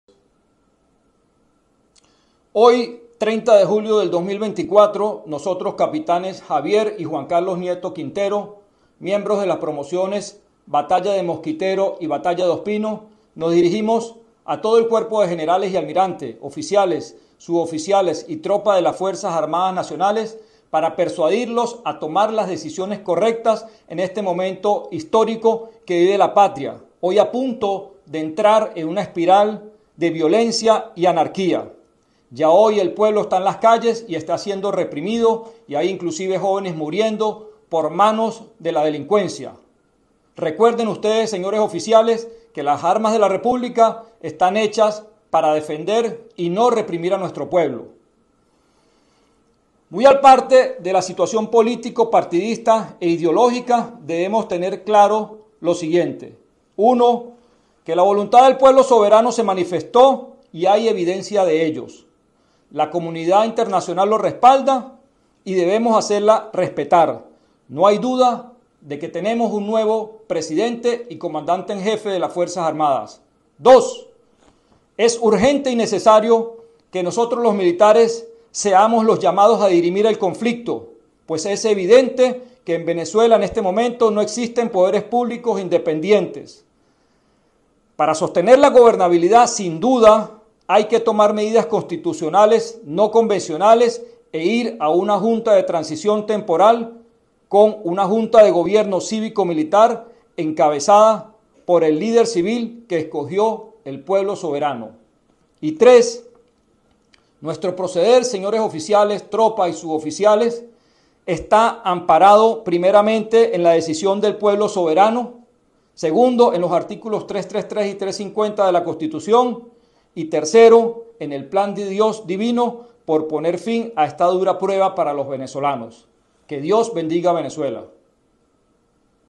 🇻🇪🚨Mensaje a las Fuerzas Armadas Venezuela Cap. Javier y Juan Carlos Nieto Q. en situación de retiro
🇻🇪🚨ATENCIÓN. Capitán Javier Nieto Quintero y Juan Carlos Nieto Quintero en situación de retiro, le envían un mensaje a la Fuerzas Armadas Venezuela,